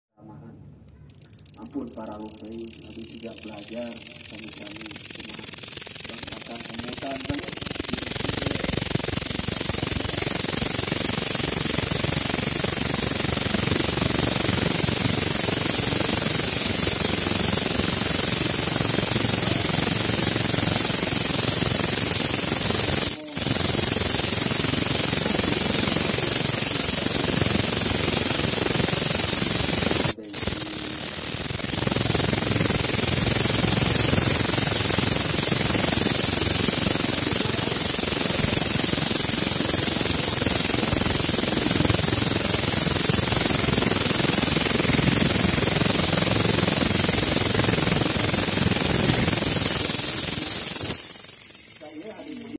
BELALANG DENGAN SUARA NYARING | CARI CANGKAS / KECEK
belalang ini identik dengan suaranya yang nyaring dan sering berbunyi di malam hari @jalanmalam7498